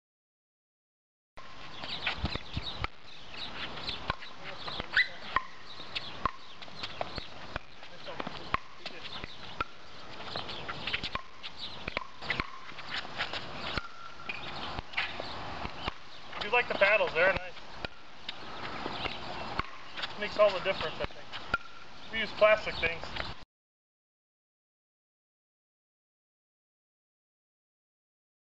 Pickleball in Peru
A group of men show off Peru's new pickleball court at 28th Street Park.